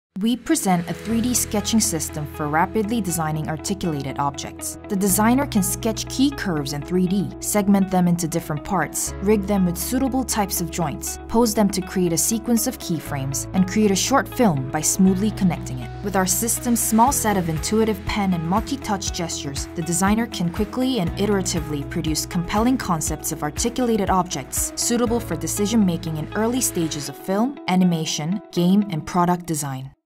Rapid design of articulated objects (SIGGRAPH 2022) - 30 sec. preview
✨ Live demo @ SIGGRAPH 2022 Emerging Technologies
Time: Mon Aug 8, 1:30pm - 5:30pm
           Tue Aug 9, 10am - 5:30pm
           Wed Aug 10, 10am - 5:30pm
           Thu Aug 11, 10am - 3:30pm
Location: West Building, Ballroom A/B

🎙️ My talk @ SIGGRAPH 2022
Time: Tue Aug 9, 2:15pm - 3:45pm
Location: West Building, Room 109-110

SIGGRAPH '22
Rapid design of articulated objects

Designing articulated objects is challenging because, unlike with static objects, it requires complex decisions to be made regarding the form, parts, rig, poses, and motion. We present a novel 3D sketching system for rapidly authoring concepts of articulated objects for the early stages of design, when designers make such decisions. Compared to existing CAD software, which focuses on slowly but elaborately producing models consisting of precise surfaces and volumes, our system focuses on quickly but roughly producing models consisting of key curves through a small set of coherent pen and multi-touch gestures. We found that professional designers could easily learn and use our system and author compelling concepts in a short time, showing that 3D sketching can be extended to designing articulated objects and is generally applicable in film, animation, game, and product design.